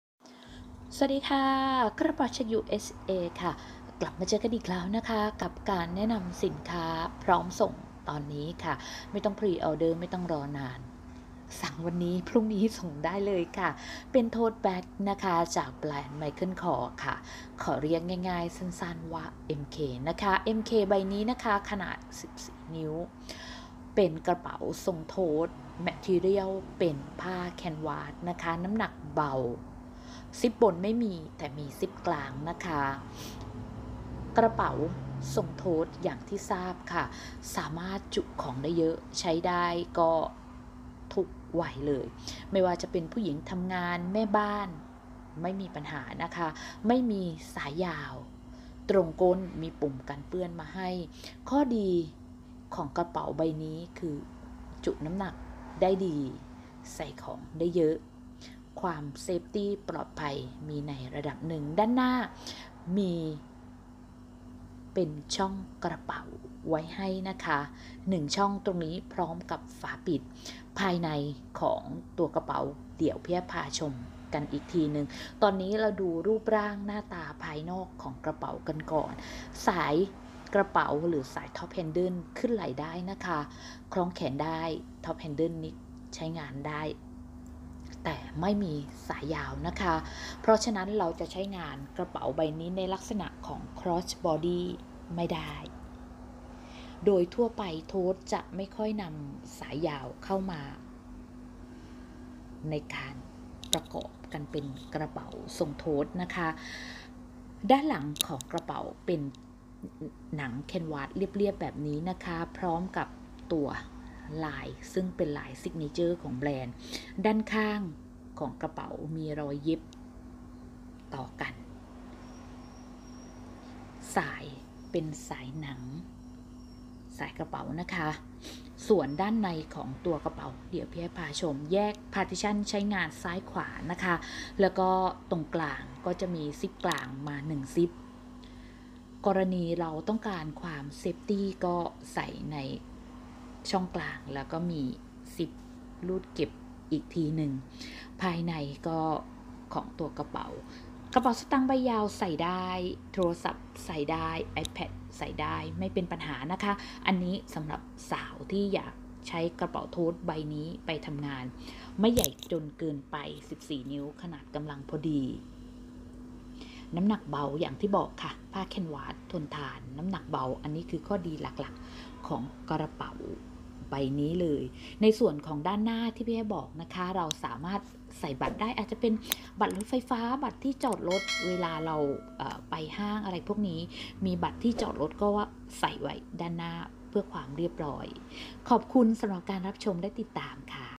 mk totebag jetsetitem สินค้าพร้อมส่ง
กระเป๋าสะพายจาก Michael kore วัสดุจากผ้า canvas น้ำหนักเบา ทนทาน ใช้งานคุ้ม